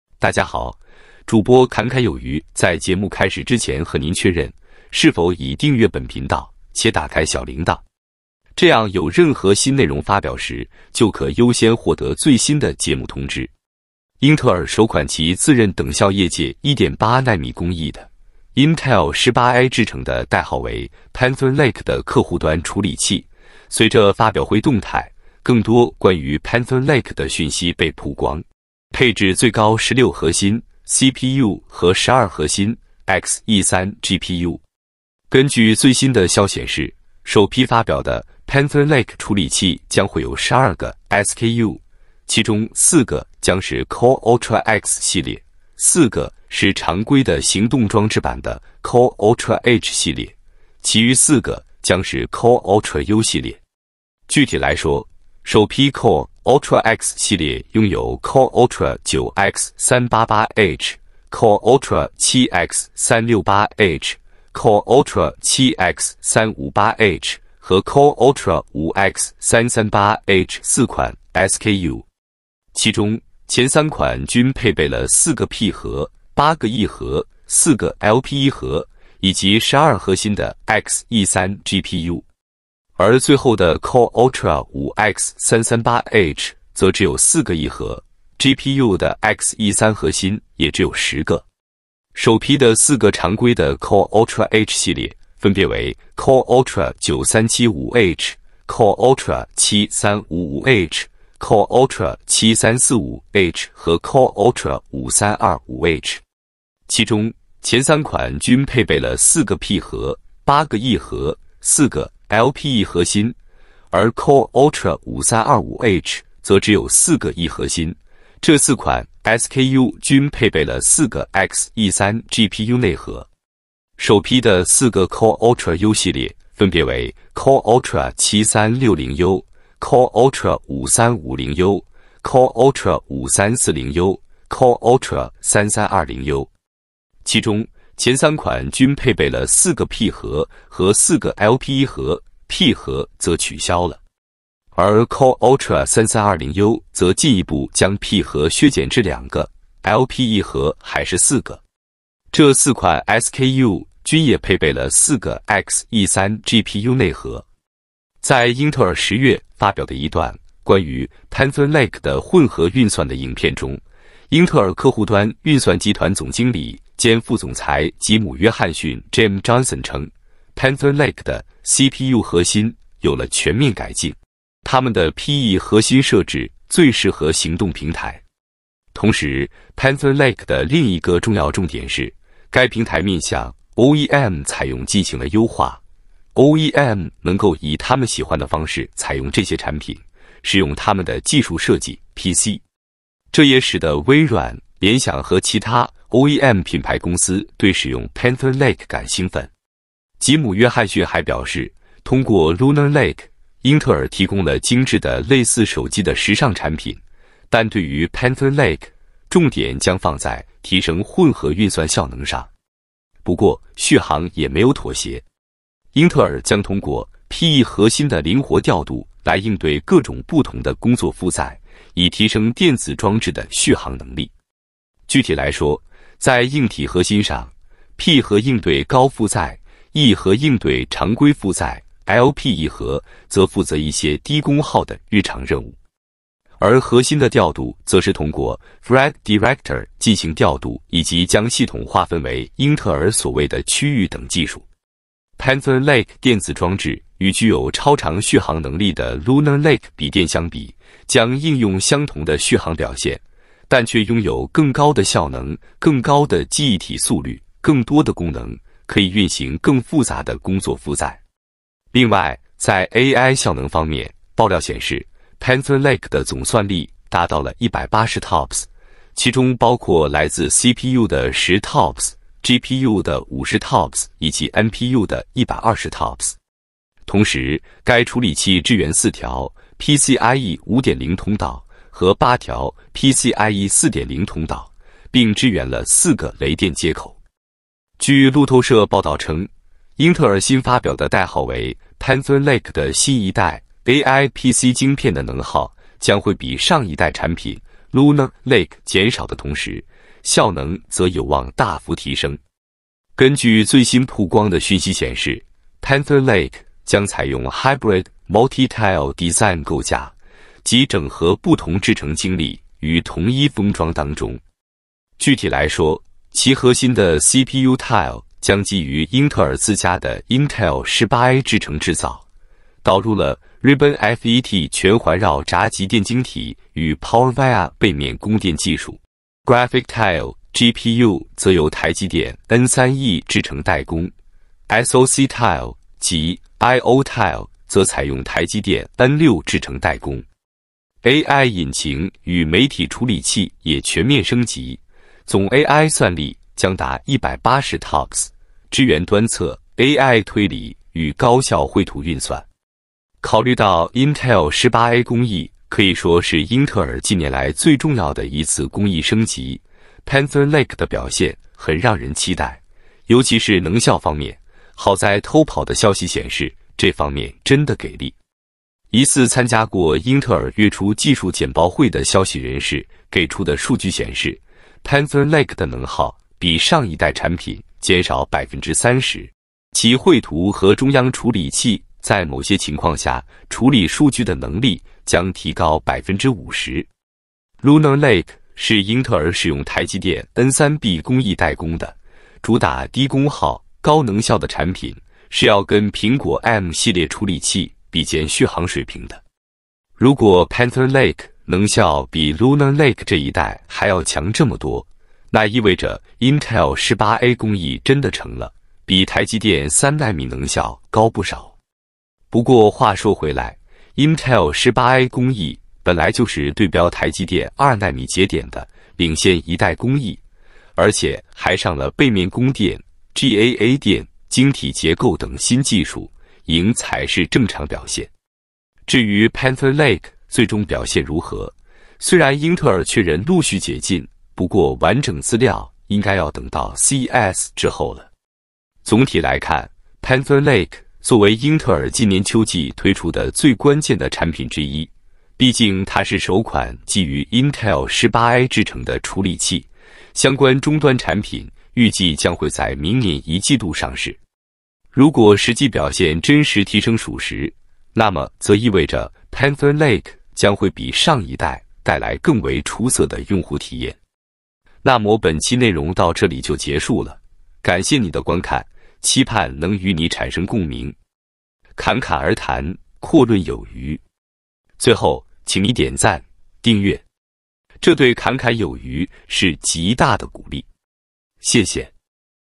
極強殺瘋！英特爾Panther Lake牙膏爆，全新CPU竟用上1.8nm提升炸裂。媒體稱完勝台積電3nm，酷睿Ultra 300處理器效能暴增，Intel 18A工藝為幾年來最重要制程升級。
在英特爾10月發表的一段關於Panther Lake的混合運算的影片中，英特爾客戶端運算集團總經理兼副總裁吉姆·約翰遜 Jim Johnson稱，Panther Lake的CPU核心有了全面改進，它們的 P/E 核心設置最適合行動平台。據路透社報道稱，英特爾新發表的代號為Panther Lake的新一代AI PC晶片的能耗將會比上一代產品 Lunar Lake減少的同時，效能則有望大幅提升。Lunar Lake是英特爾使用台積電N3B工藝代工的，主打低功耗、高能效的產品，是要跟蘋果M系列處理器比肩續航水平的。如果Panther Lake能效比Lunar Lake這一代還要強這麼多，那意味著Intel 18A工藝真的成了。